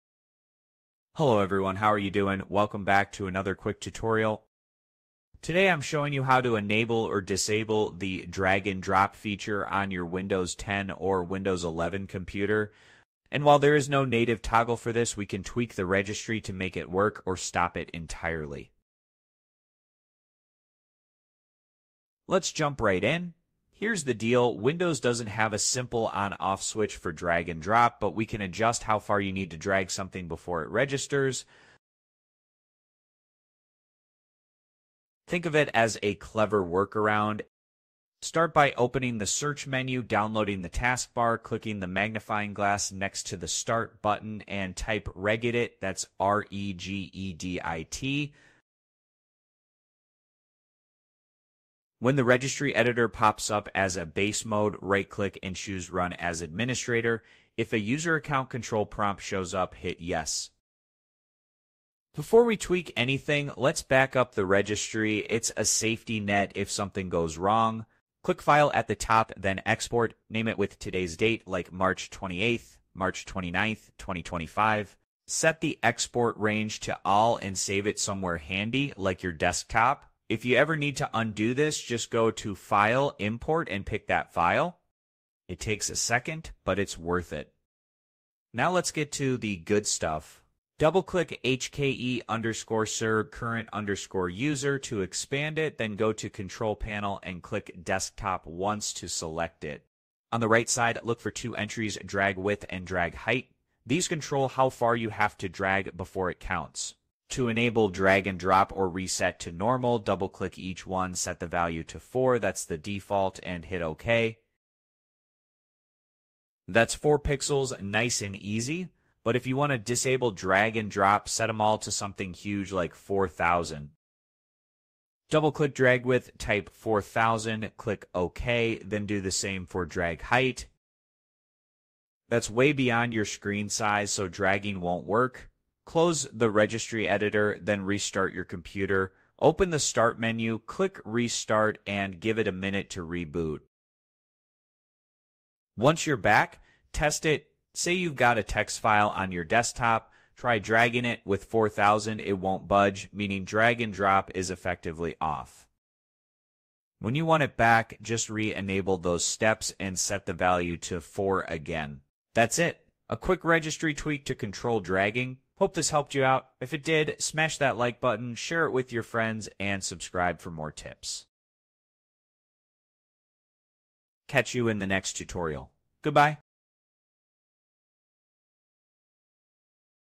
How to Fix Drag and Drop Not Working In Your Windows 11 [Solved]
Dragging and dropping files is a useful feature, but sometimes you might want to disable it—especially if you're experiencing accidental moves. In this tutorial, we’ll show you how to enable or disable drag and drop by tweaking the Windows Registry Editor. No extra software required!

🔹 What You’ll Learn in This Video:
✅ How drag and drop works in Windows 10 & 11
✅ How to tweak Registry Editor to disable or enable drag and drop
✅ The best values to set for customizing drag sensitivity

📌 Timestamps:
⏱️ 00:00 - Introduction
⏱️ 00:38 - Opening the Registry Editor (regedit)
⏱️ 01:02 - Creating a backup before making changes
⏱️ 01:28 - Navigating to the correct registry path
⏱️ 01:40 - Modifying dragWidth and dragHeight

Many users find drag and drop too sensitive, leading to accidental file moves. This tutorial offers a simple workaround using Registry Editor—allowing you to either disable or fine-tune drag sensitivity. By the end, you'll have complete control over drag and drop on your Windows PC!